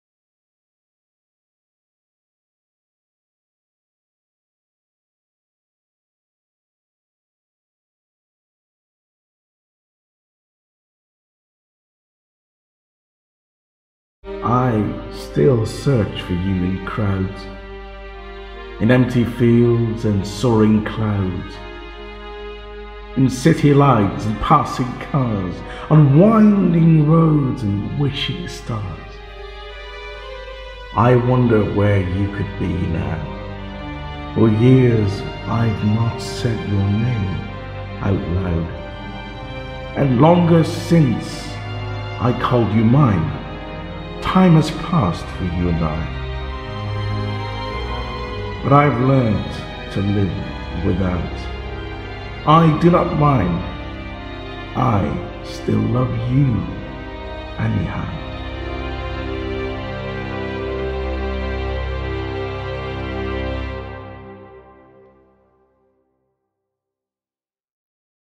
Wishing Stars - Lang Leav
A poem from "Lullabies" (2014) by Lang Leav. Music & recitation by Charles Belfor.